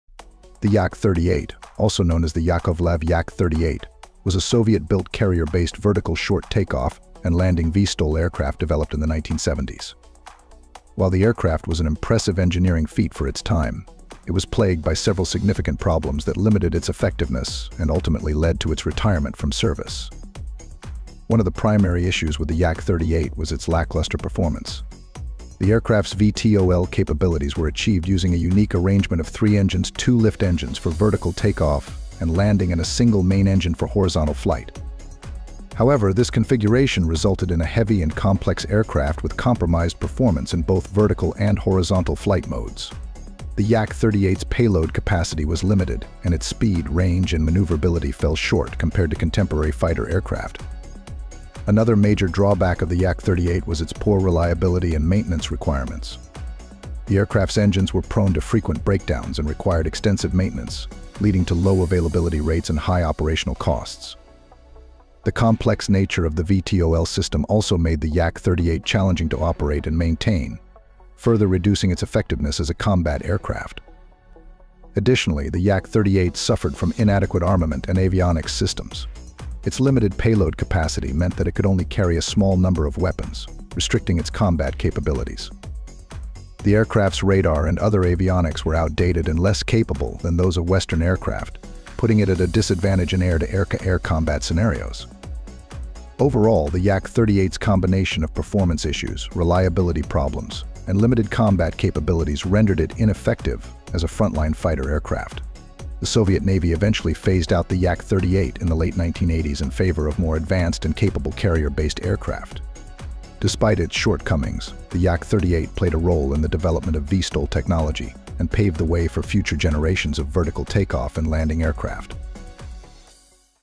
What was the problem with the Yak-38?
Soviet designers went with the Tumansky R-28 engine, and RD-38 turbojets provided the initial lift. The Yak-38 did not have impressive specs. Its top flight speed was 795 miles per hour, with a ceiling of 36,000 feet and a range of 800 miles. They later tested it on the ground in Afghanistan, and the engines stirred up dust and debris that could obstruct the engines and stall the airplane.